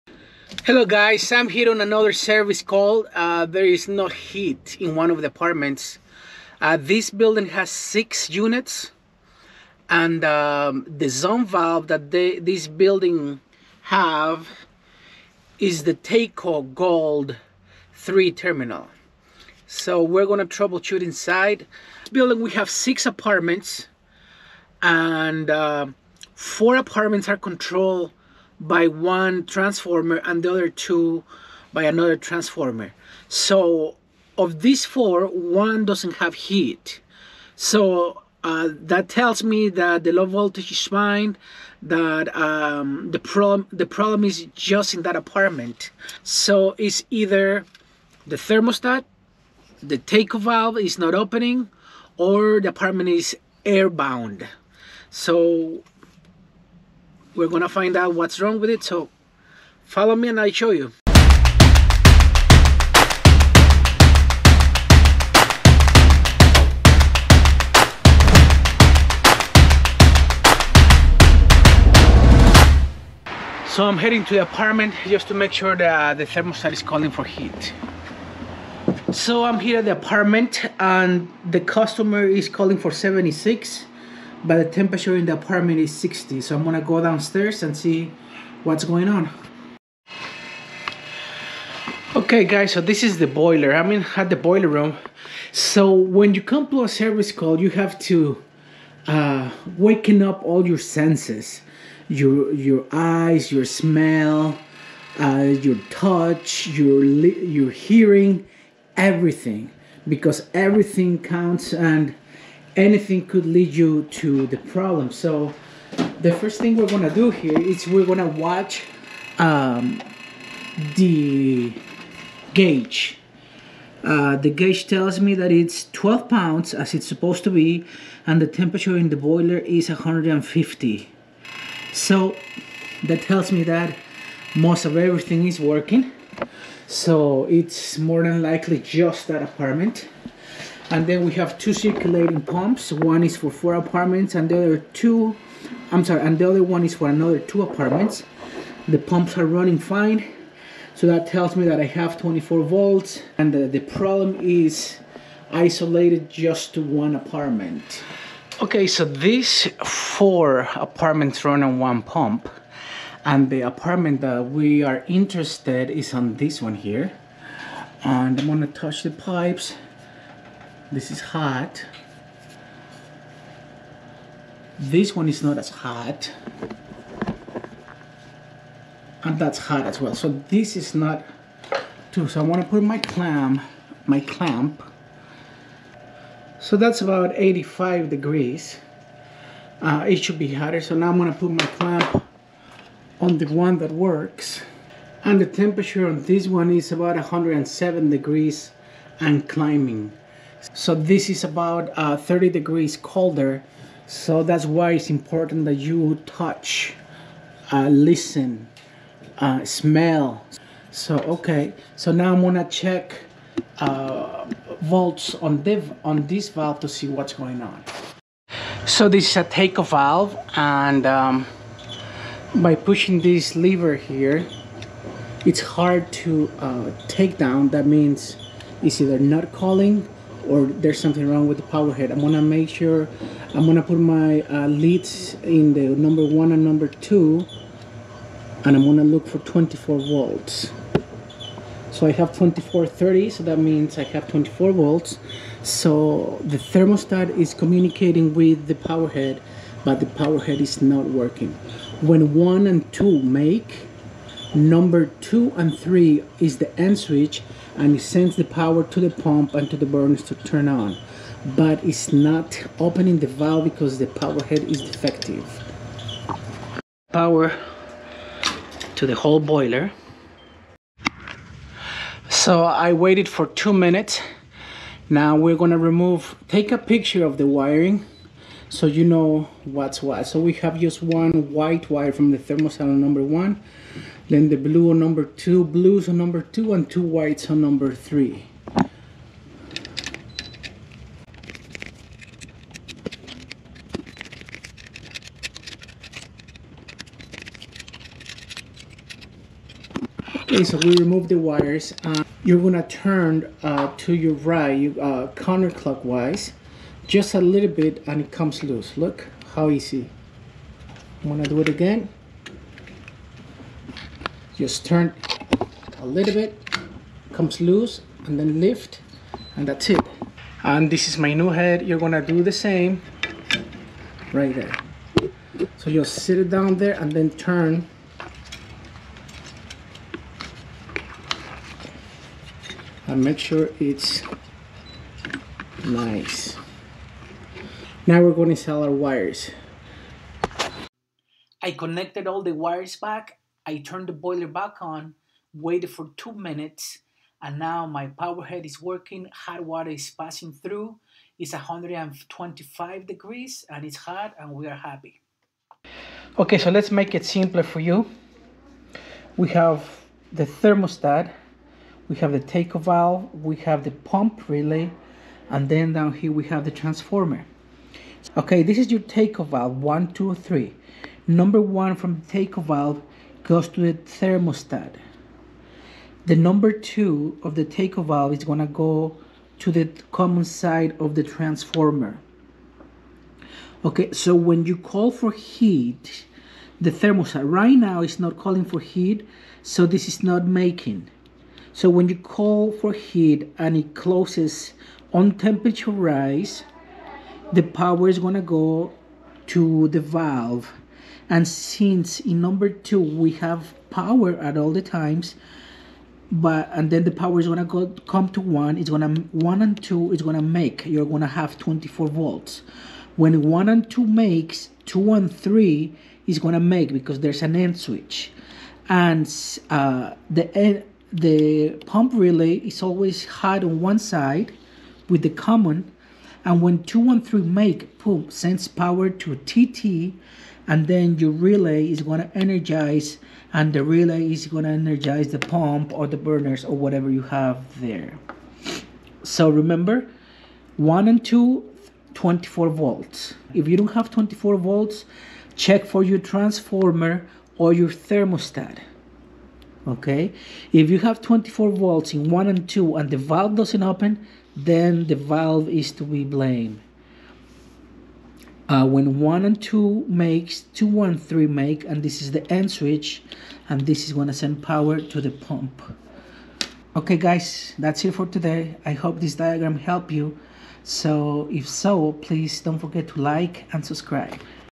NO HEAT. SERVICE CALL # 5 3 WIRE TACO VALVE FAILED. TROUBLESHOOTING
In this video I will show you how to troubleshoot and replace a taco valve on a boiler system.